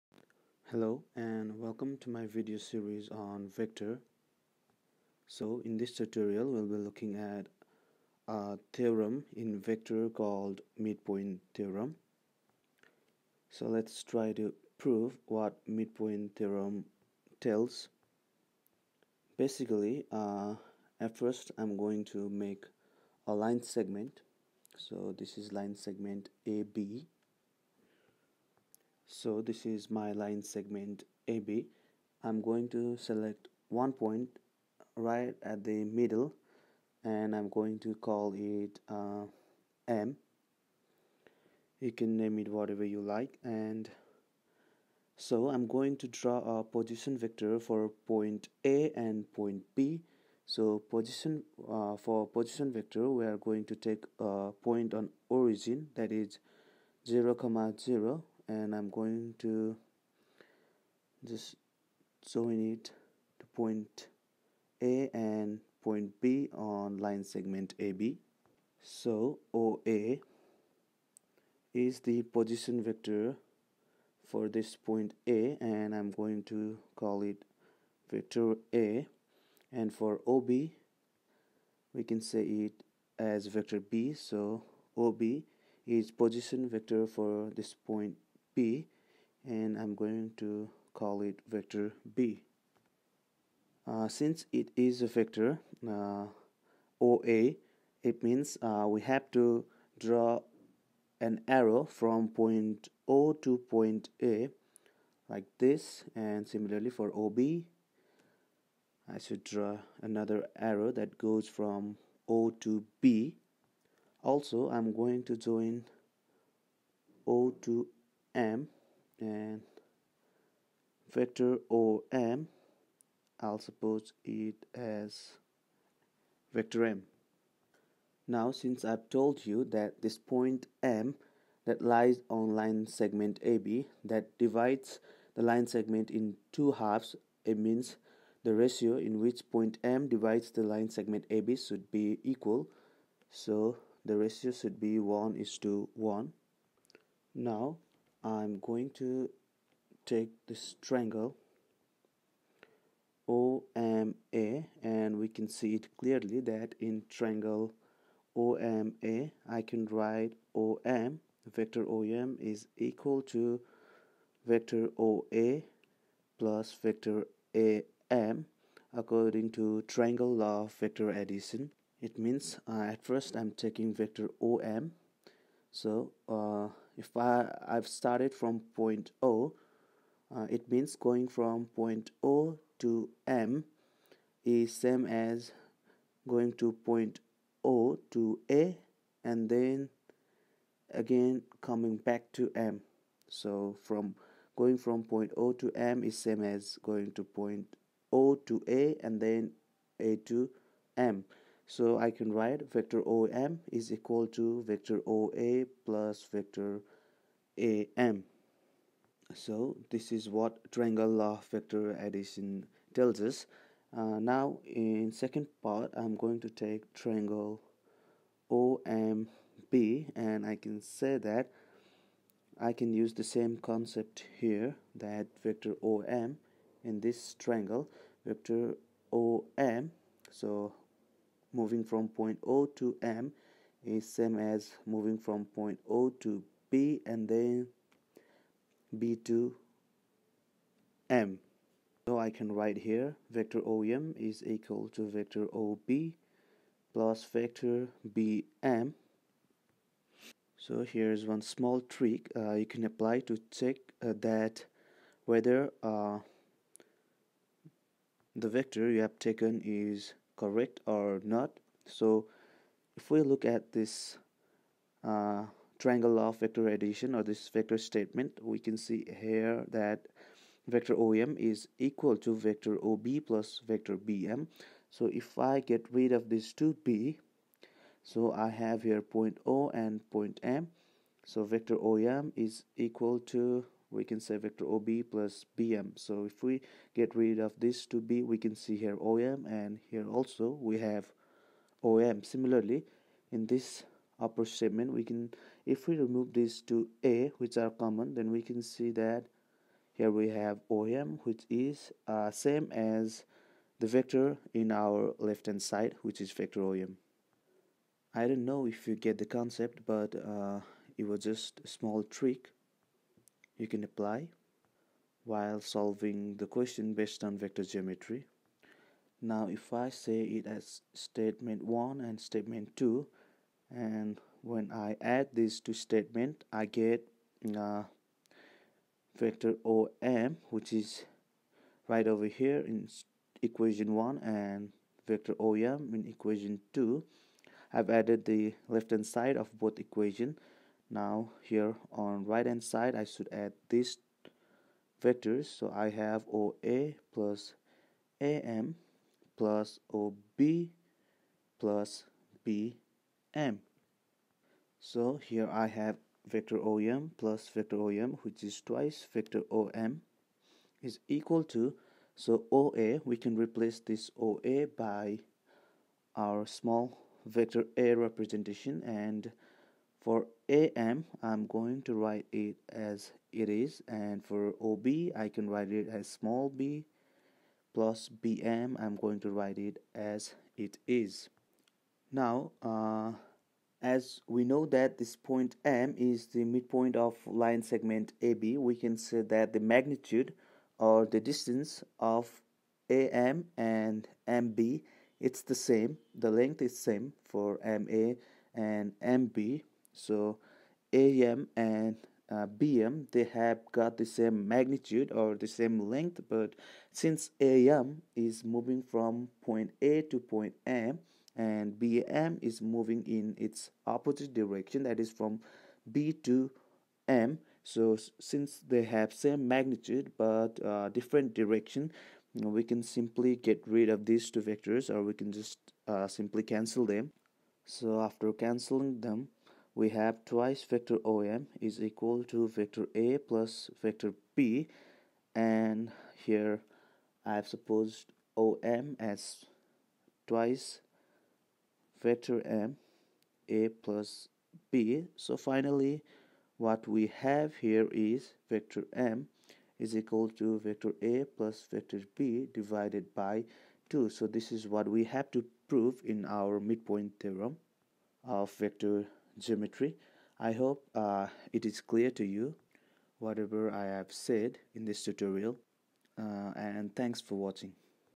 Mid point Vector theorem proof question for SEE and grade 10 explained!
Midpoint theorem Easy tips and explanation on Vector theorem for grade 10